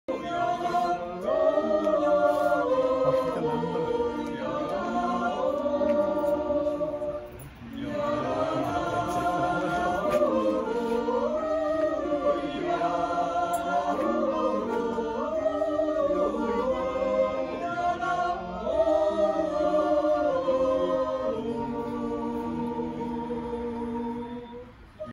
Aphasie Jodelchor Auftritt1 Dez 2021
Der Aphasie Jodelchor durfte im Dezember 2021 seinen ersten Auftritt geniessen.